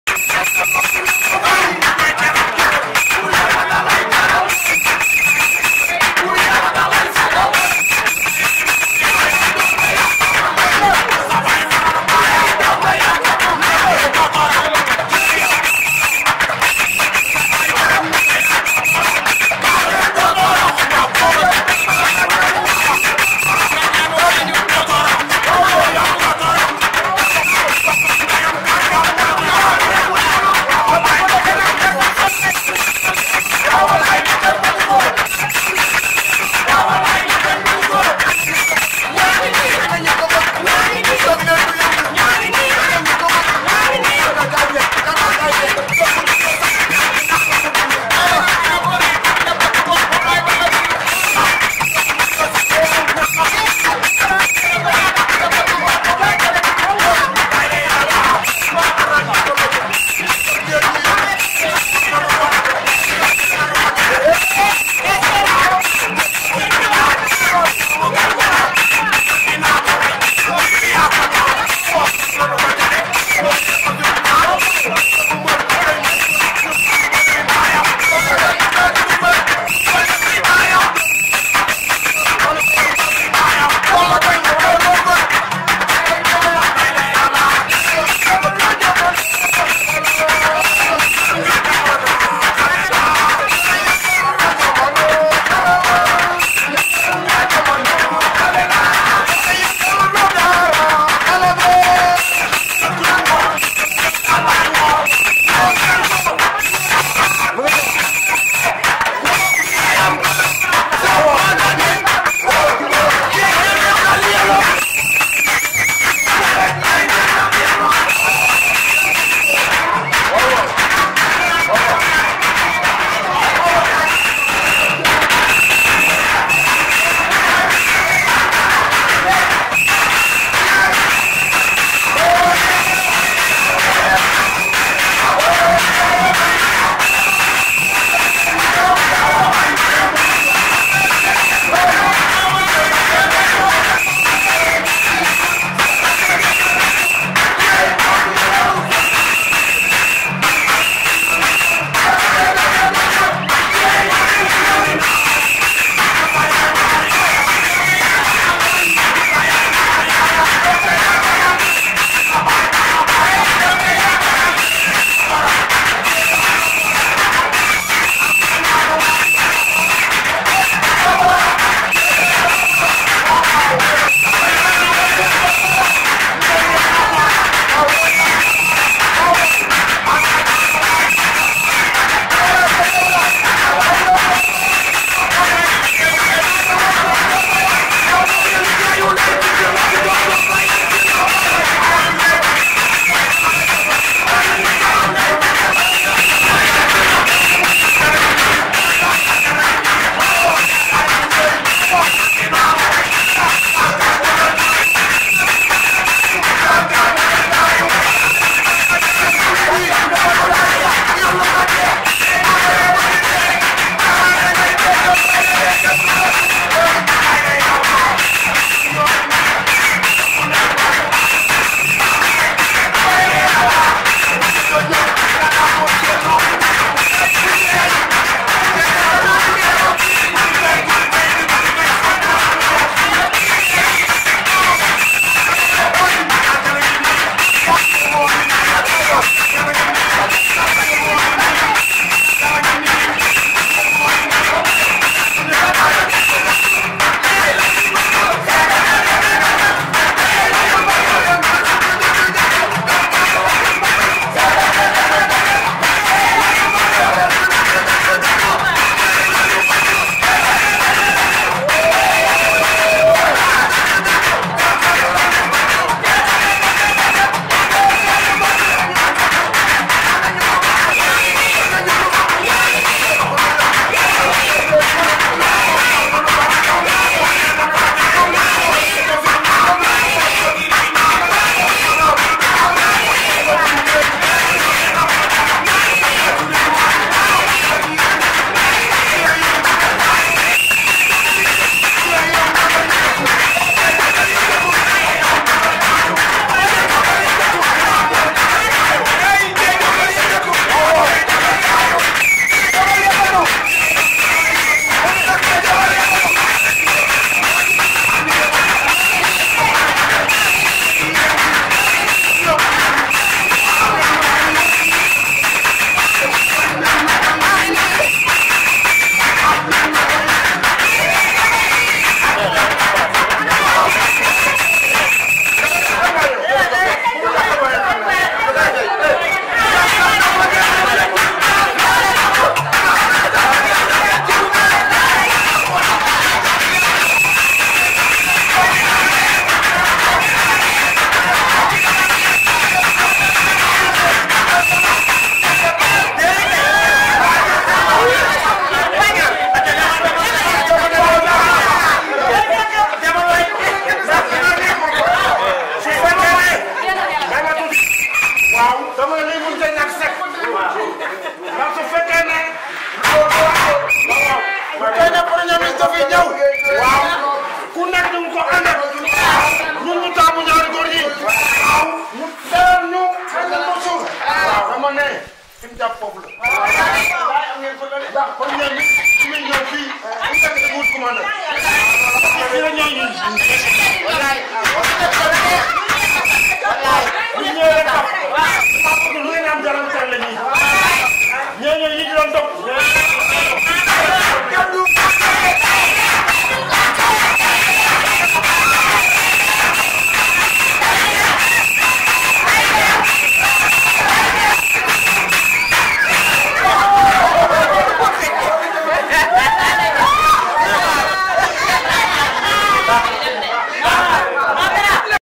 ALIOUNE DIEYE ET GANKAL KA EN FÊTE APRES L'ARRIVEE DU PM A DAHRA
NE RATEZ SURTOUT PAS CETTE ANIMATION EXTRAORDINAIRE.
Surtout ne dansez pas.